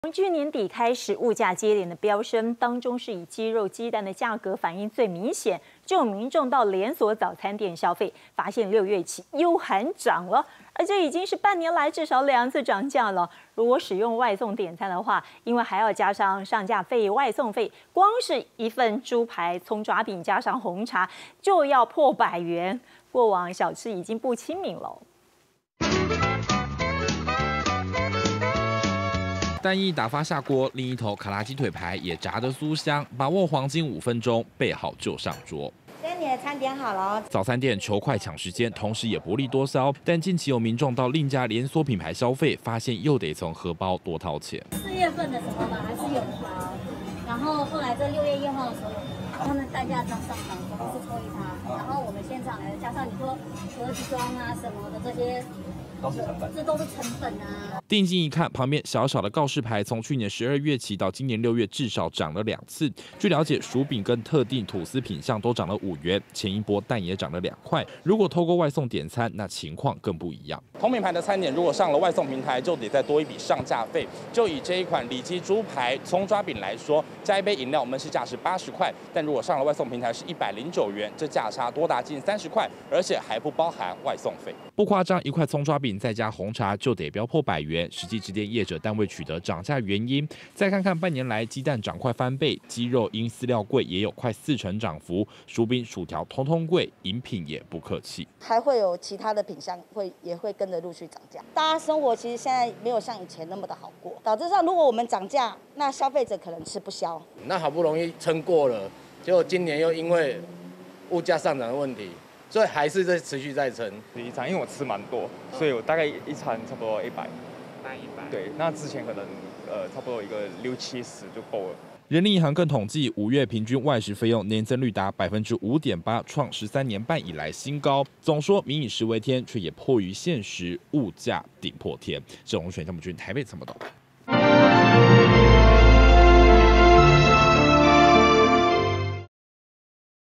半年漲3波？ 早餐店消費驚見「6月又喊漲」 民眾嘆：小吃不再親民
從去年底開始物價接連飆升，當中以雞肉、雞蛋的價格反應最明顯。就有民眾到連鎖早餐店消費，發現6月起又喊漲，而這已經是半年來至少2次漲價，如果使用外送點餐，因為還要加上上架費、外送費，光一份豬排蔥抓餅加上紅茶要價破百，過往小吃已經不親民。